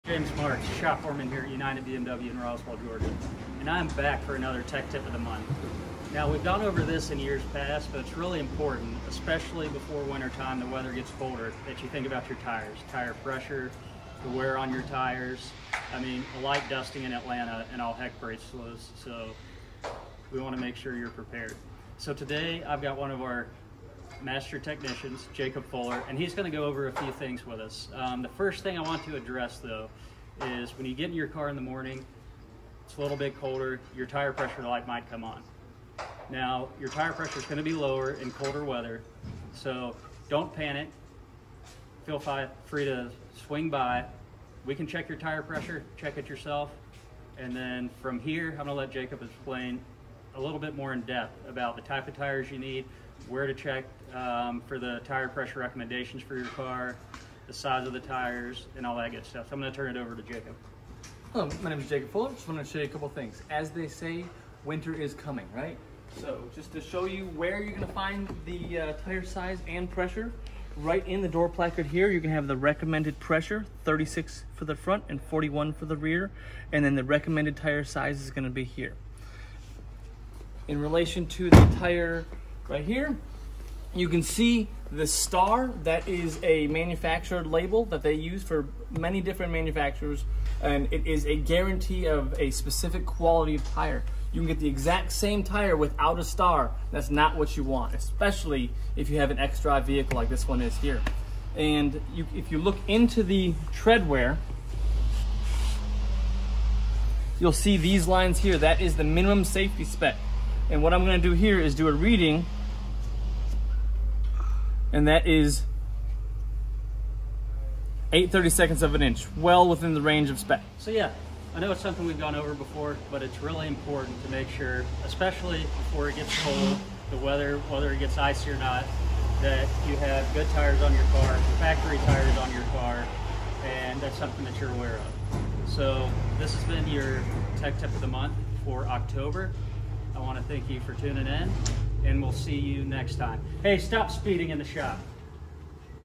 BMW Tech Tip of the Month: Tires
Trick or Treat? How about a tip? Our tech tip of the month is all about tires.